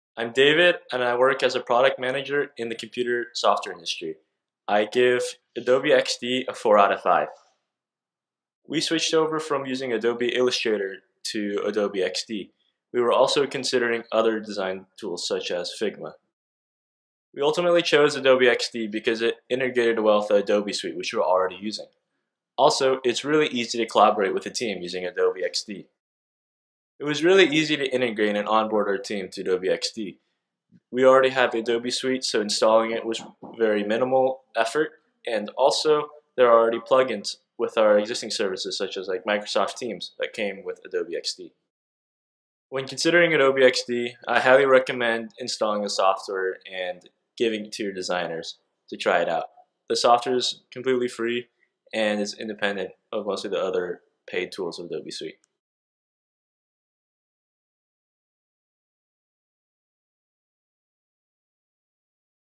Adobe XD Review: Most simple UI/UX tool out there
Adobe XD Review.

On-premise UX tool for businesses that helps create designs for web and mobile via prototyping, sharing, collaboration, and more.